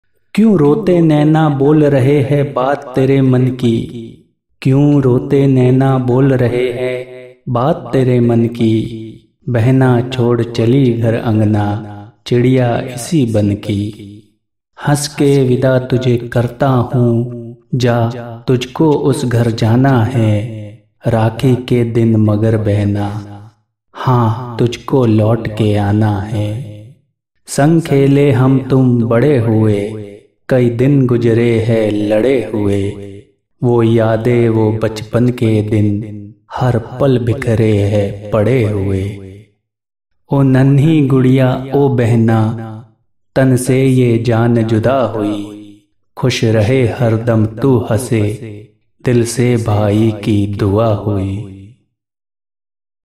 बहन की विदाई शायर || Sister Marriage Shayari || Bahan shayari || बहन की विदाई शायरी शादी के लिए
बहन की विदाई शायरी शादी के लिए || Sister Marriage Shayari || Bahan shayari

❖ Album          : Behan ki shayari
❖ Writer         : Hitesh Choudhary
❖ Voice          : Hitesh Choudhary
❖ Category       : sister marriage shayari
❖ Sub Category   : hindi shayari